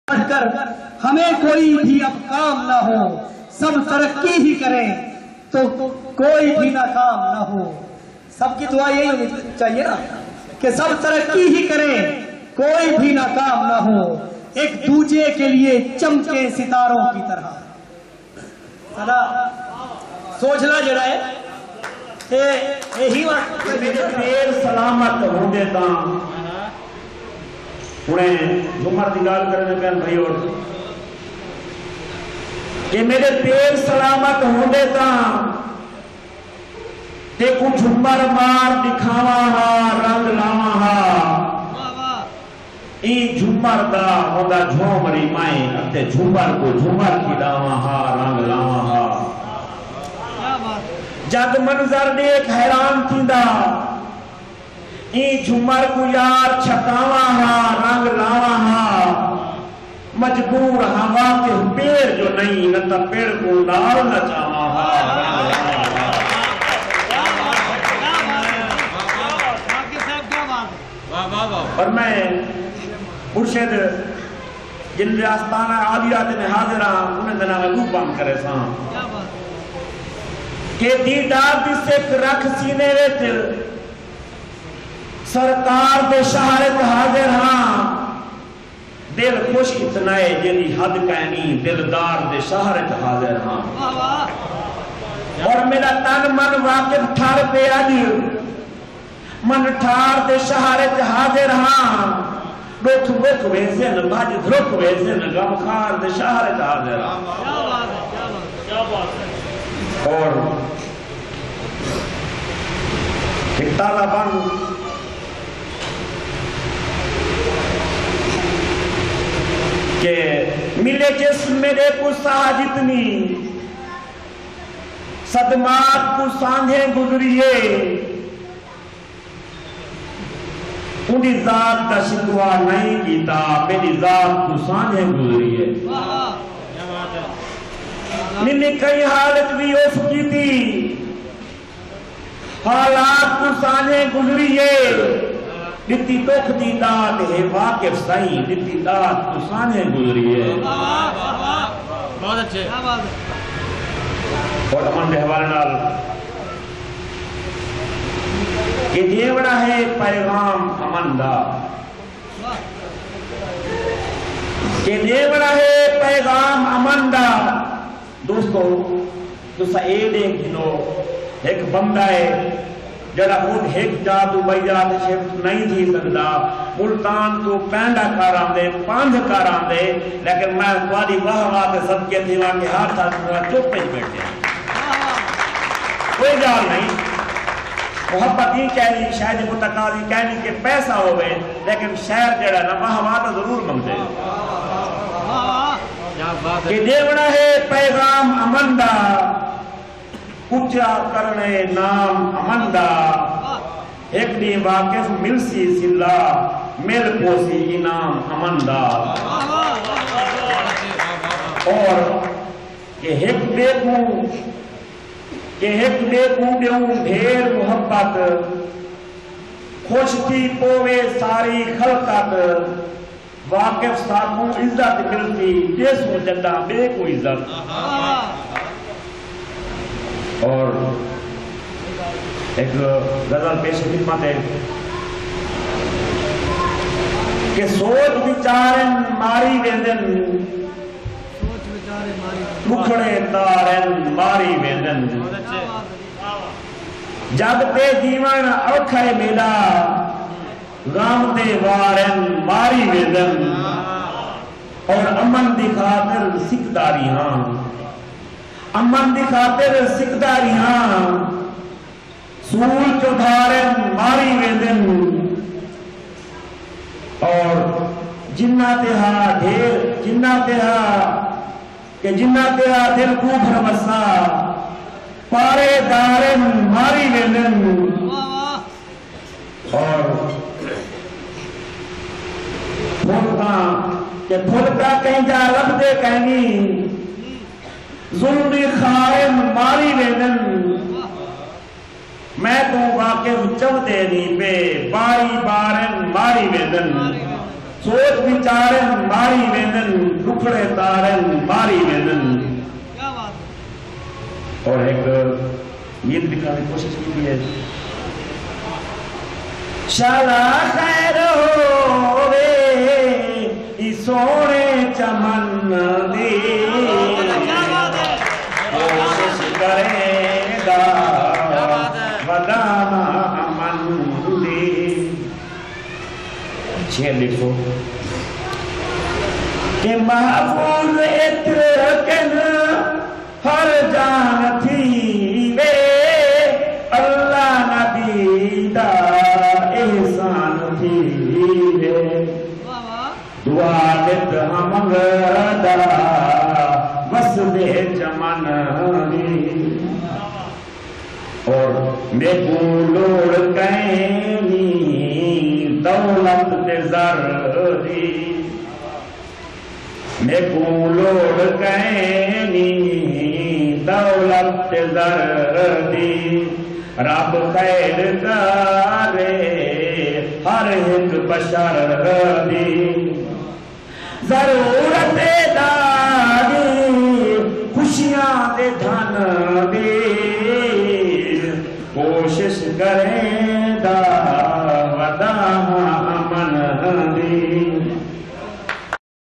Waseb Aman Mushaira (Part - 4) Waqif Multani
Waseb Aman Mushaira is an activity organized by Sojhla for Social Change for the promotion of peac. The activity was organized at Makhdoom Rasheed. The activity was participated by poets of south Punjab. Waqif Multani presented his poetry  in this Mushaira.